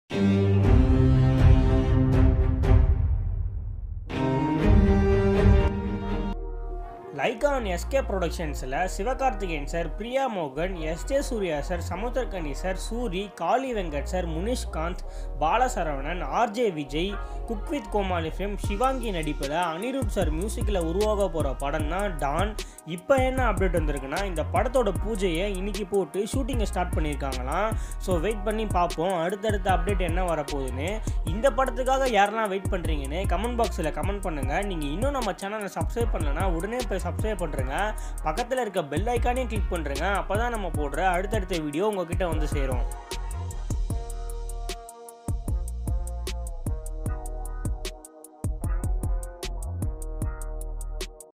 DON movie shoot updates | Sivakarthikeyan | Priyamohan | Sivaangi | BWC
This video about sivakarthikeyan,priyamohan,sivangi,soori next don movie updates and details .